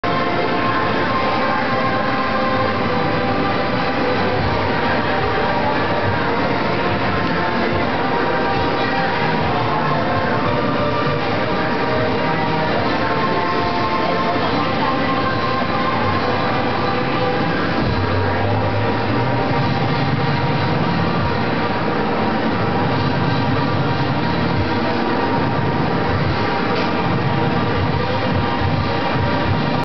Japanese Girl Dancing outside Pachinko Palace - 2007
Osaka Japan - 2007